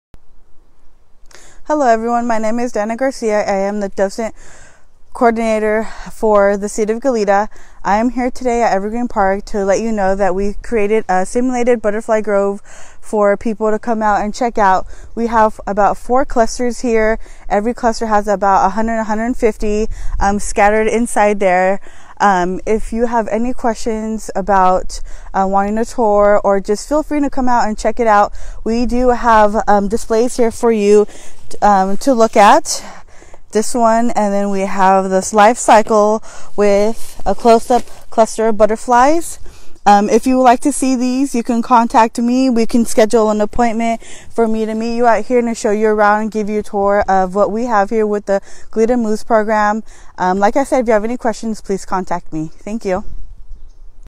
Behind the scenes of the Goleta MOVES Educational Program
Behind the scenes of the Goleta MOVES Educational Program at Evergreen Park! MOVES is the Monarch Overwintering Visual Experience Simulation.